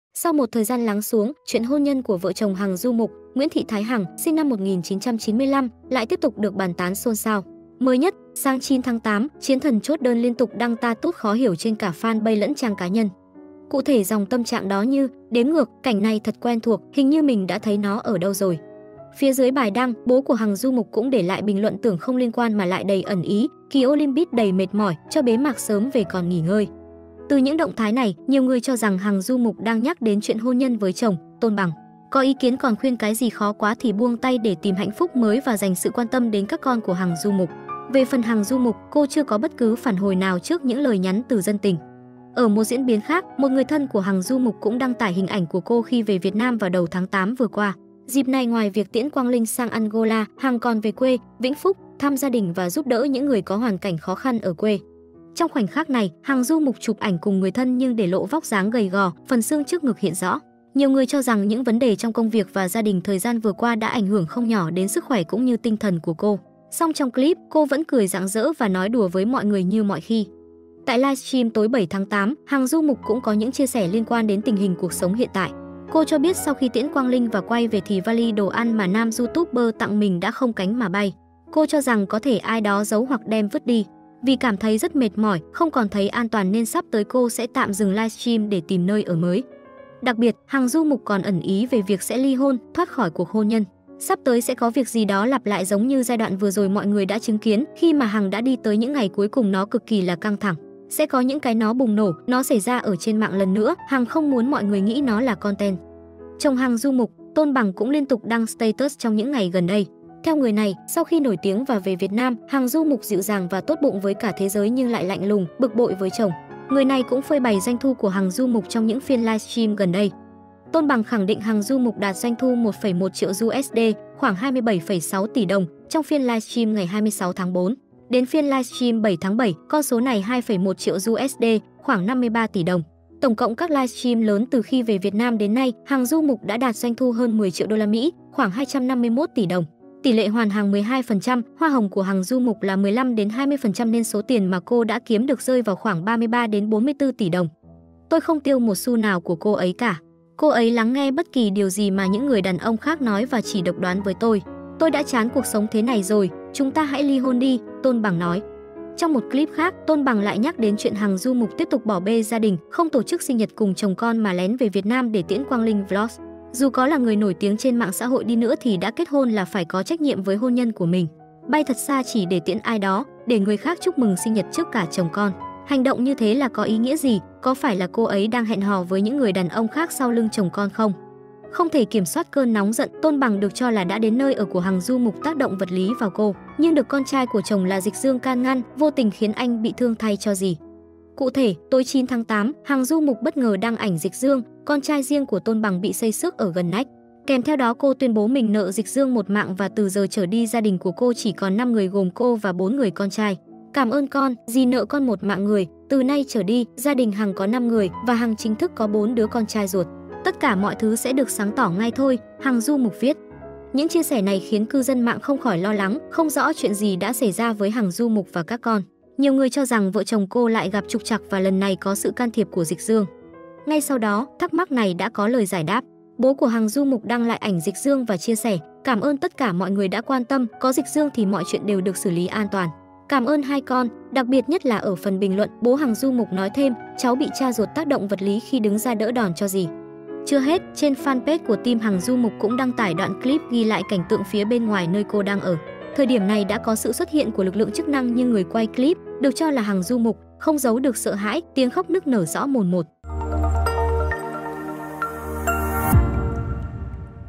Hằng Du Mục cầu cứu CĐM, chồng mất kiểm soát?
Sau một thời gian lắng xuống, chuyện hôn nhân của vợ chồng Hằng Du Mục (Nguyễn Thị Thái Hằng, sinh năm 1995) lại tiếp tục được bàn tán xôn xao. Mới nhất - sáng 9/8, “chiến thần chốt đơn” liên tục đăng status khó hiểu trên cả fanpage lẫn trang cá nhân

Cụ thể dòng tâm trạng đó như: “Đếm ngược!”, “Cảnh này thật quen thuộc, hình như mình đã thấy nó ở đâu rồi”.
Phía dưới bài đăng, bố của Hằng Du Mục cũng để lại bình luận tưởng không liên quan mà lại đầy ẩn ý: “Kỳ Olympic đầy mệt mỏi, cho bế mạc sớm về còn nghỉ ngơi”.
Từ những động thái này, nhiều người cho rằng Hằng Du Mục đang nhắc đến chuyện hôn nhân với chồng - Tôn Bằng. Có ý kiến còn khuyên cái gì khó quá thì buông tay để tìm hạnh phúc mới và dành sự quan tâm đến các con của Hằng Du Mục. Về phần Hằng Du Mục, cô chưa có bất cứ phản hồi nào trước những lời nhắn từ dân tình.